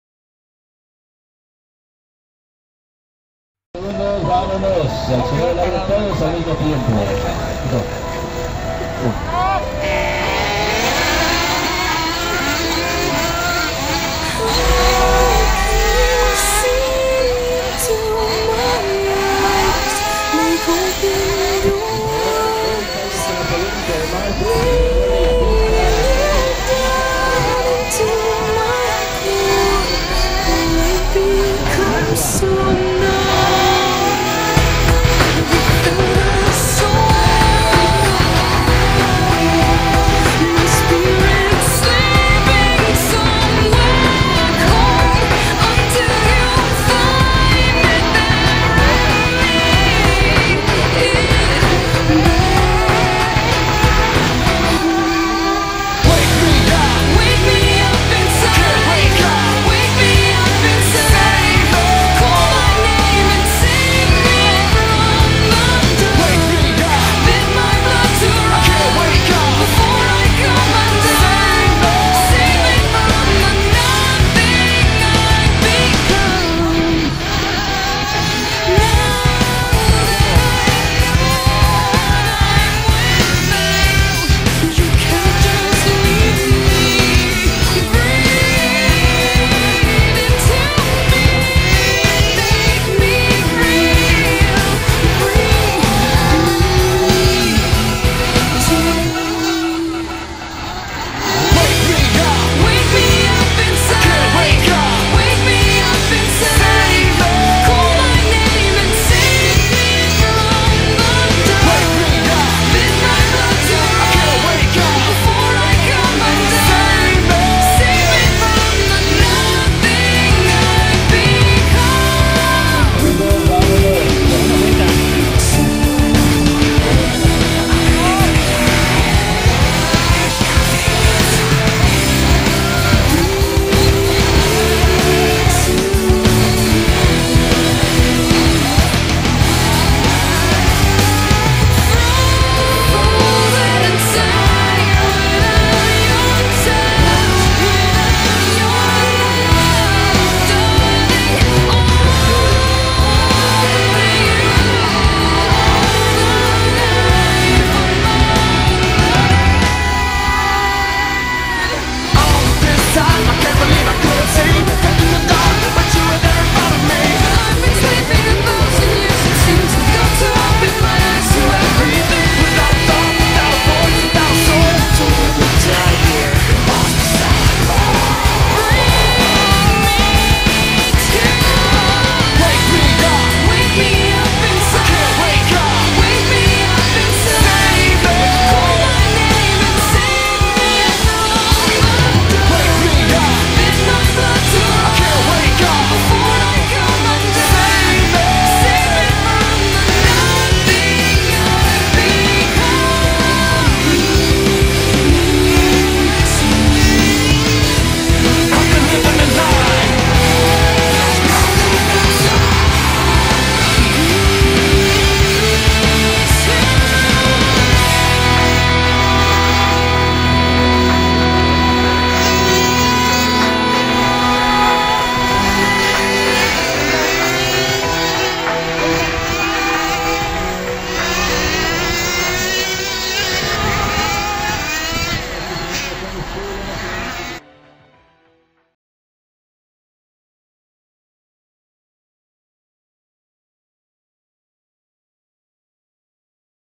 Rally Game Mundo E Agosto 25 inferno GT KAORI
Babani, Erpel, Montaño, Patiño, Ruiz y Velazquez (Chucho y Ricardo), una carrera reñida, disfrútenla!!